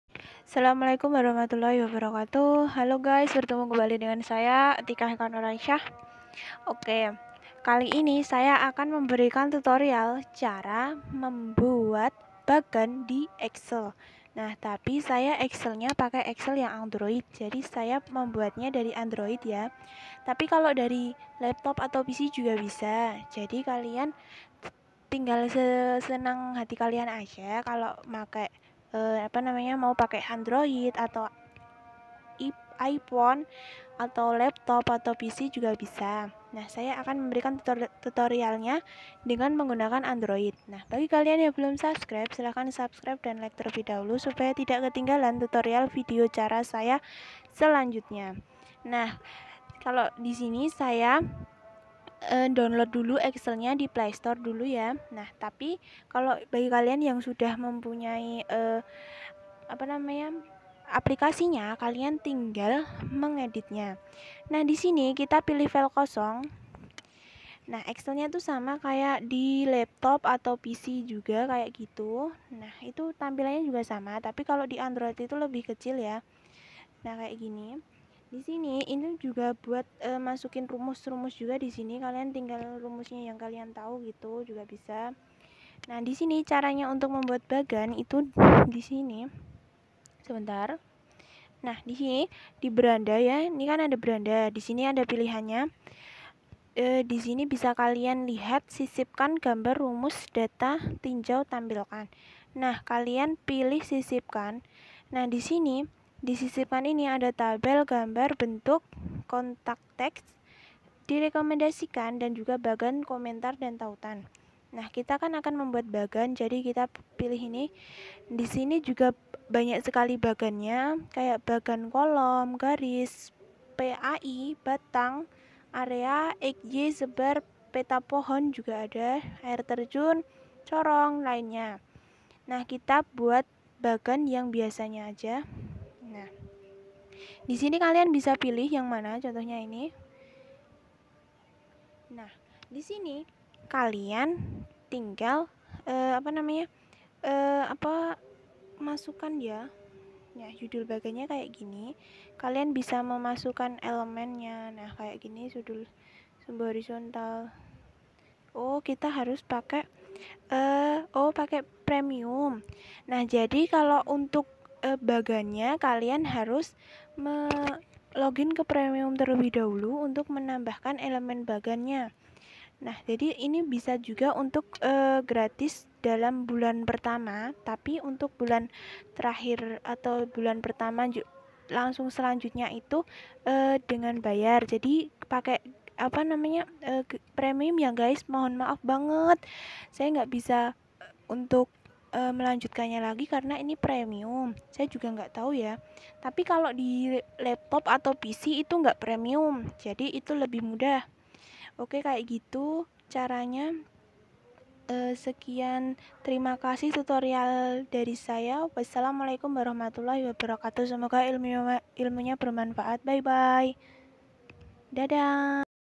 Cara Membuat Bagan Di Excel
kalian bisa membuka excel kalian dari android,pc,laptop.kalau saya memberikan tutorialnya dari android nah kalian bisa buka excel lalu untuk lebih lanjut kalian bisa tonton video saya diatas.
SELAMAT MENCOBA
BYE BYE